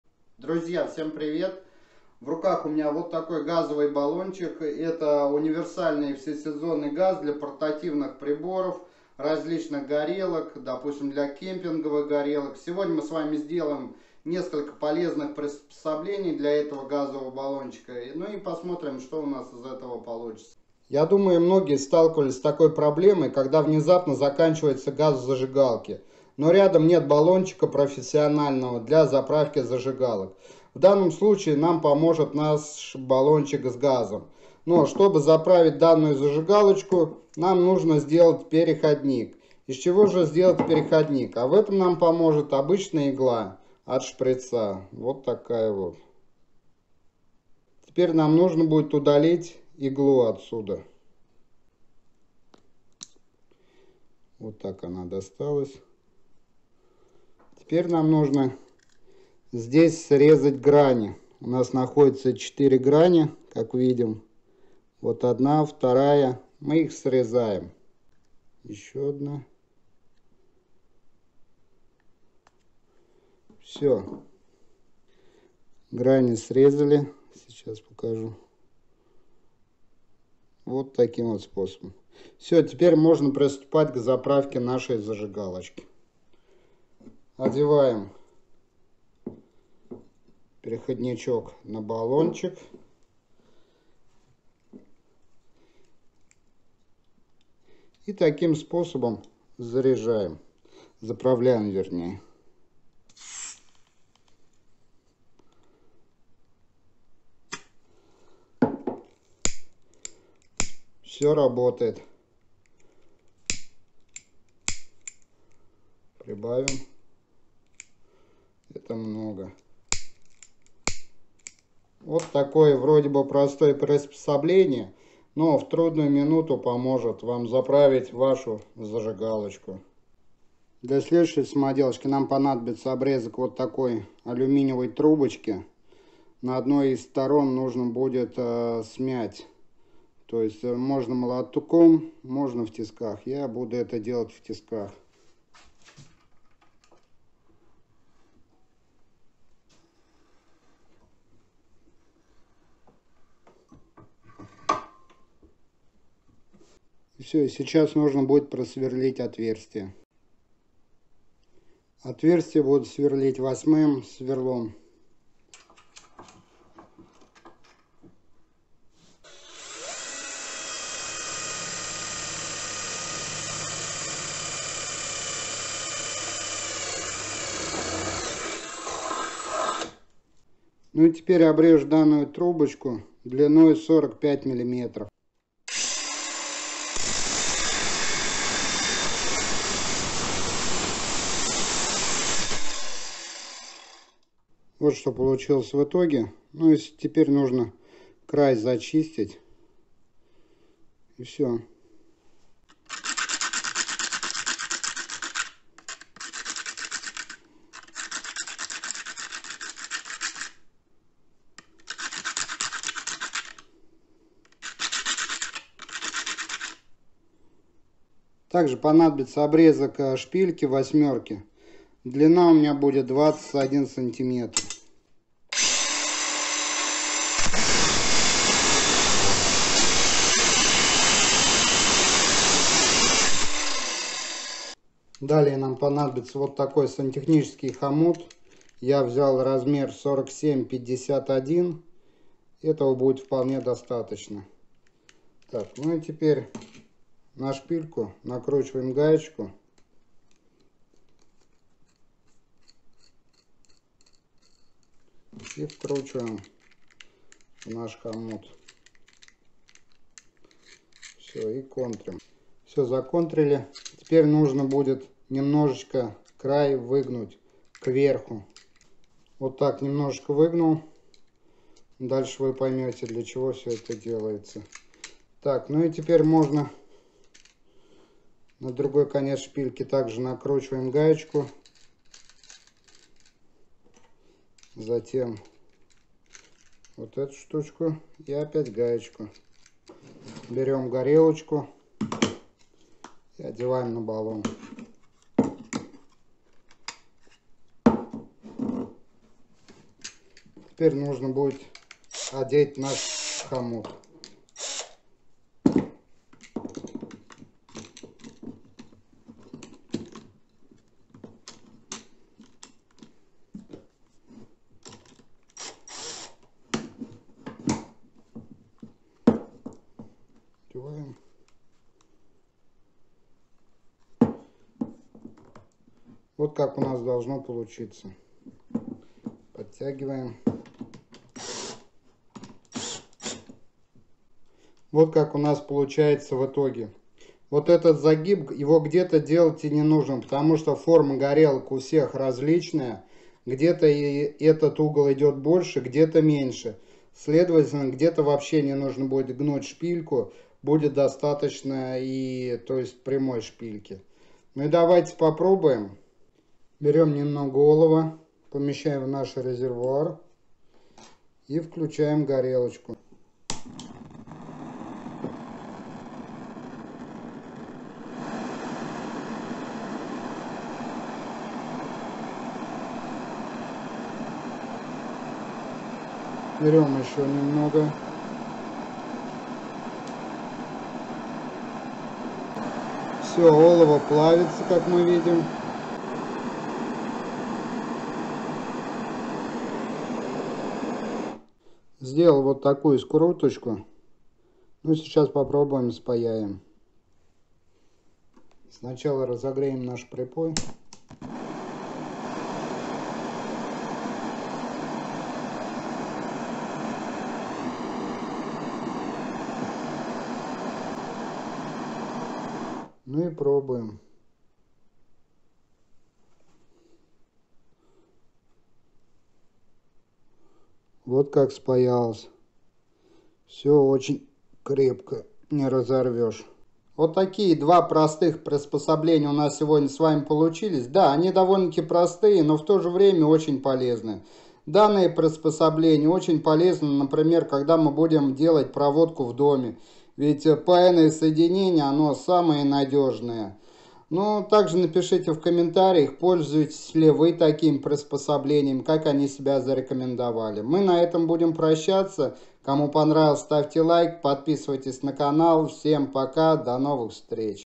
Мало кто знает об этой функции ХОМУТА!!!
КЛАССНАЯ и ПРОСТАЯ ИДЕЯ из газового баллончика! Полезная самоделка своими руками! Девайс! Для гаража